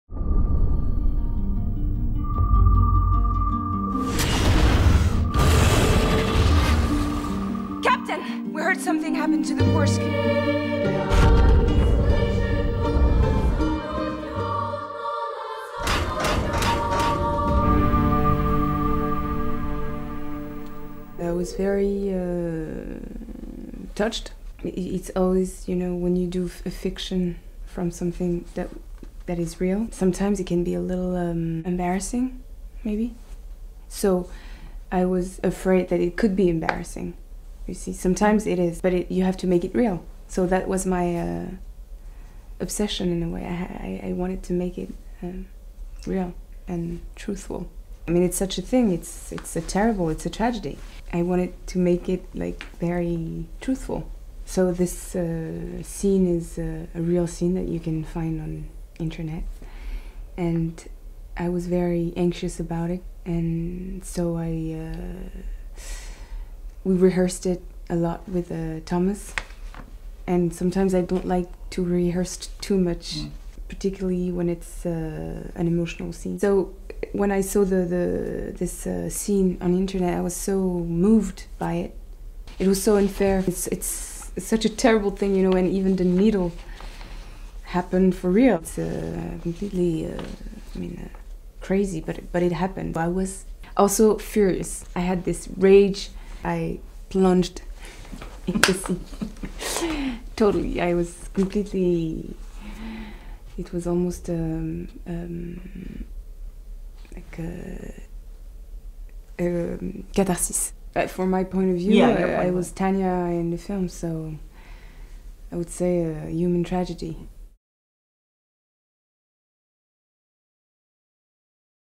KURSK Interview Highlights - Lea Seydoux 蕾雅絲端訪問
我十分感動
當你要演繹一個基於真實改編的人物
有時候可能會有點尷尬
因此我開始時有點怕尷尬
有時候會這樣，但你必須演得真實
因此這就是我的執著
我希望能真實且忠實地演繹出來
這種事情很悲哀，是一齣悲劇
我希望能非常忠實地表現出來
因為我擁有女性一切的特質
我有目標，有自己的理由
我們有一場戲，你可以在網上找到
我曾一度擔憂
因為當時仍在拍攝中
湯瑪士要我們反覆排練
有時候我不喜歡排練得太多
尤其是感情戲
當我在網上見到這場戲之後，為之感動
這件事極不公平，非常悲哀
每個細節都曾真實發生
根本是豈有此理，但它的確發生了
我當時怒不可遏
我大發雷霆，完全投入其中
我當時完全…已經進入了角色
以我之見，作為我在電影中的角色坦雅
我會說，這是人性的悲劇